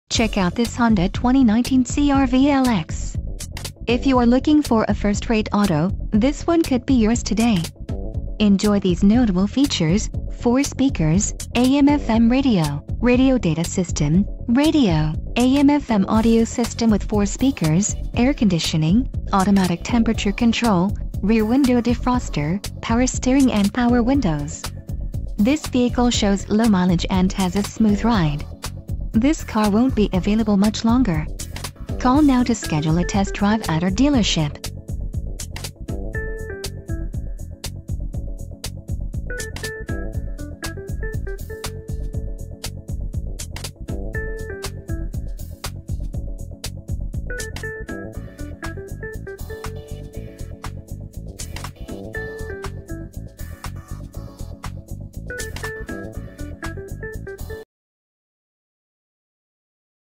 2019 Honda CR-V LX
2019 Honda CR-V LX Obsidian Blue AWD CVT 2.4L I4 DOHC 16V i-VTEC

25/31 City/Highway MPG

Located in Prescott AZ, and also serving Flagstaff, Cottonwood, Sedona, Camp Verde, Scottsdale, and the greater Phoenix area. Also, we're part of the Valley Honda Dealers.